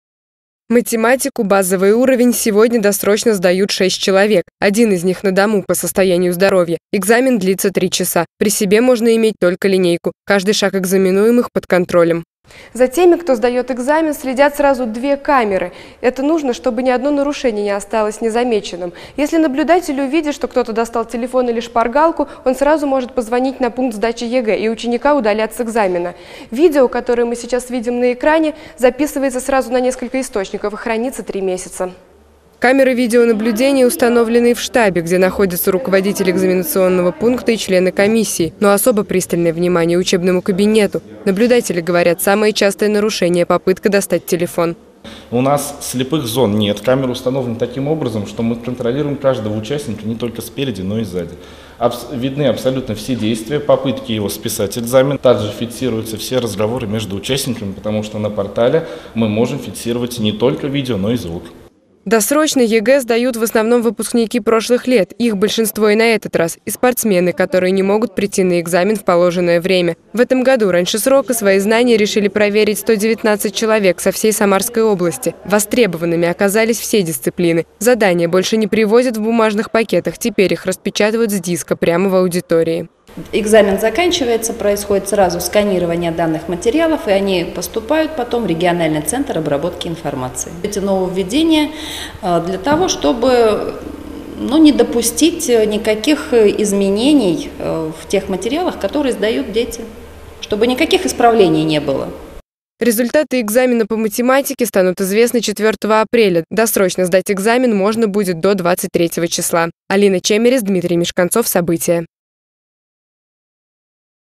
В Самарской области стартовал досрочный этап сдачи ЕГЭ
Сдать раньше срока. В Самарской области стартовал досрочный этап государственной итоговой аттестации. Первый экзамен по математике прошел в школе №43. ЕГЭ сдавали 6 человек. Чтобы не было нарушений, за всеми, кто садится за парту, с помощью специального интернет-портала следят наблюдатели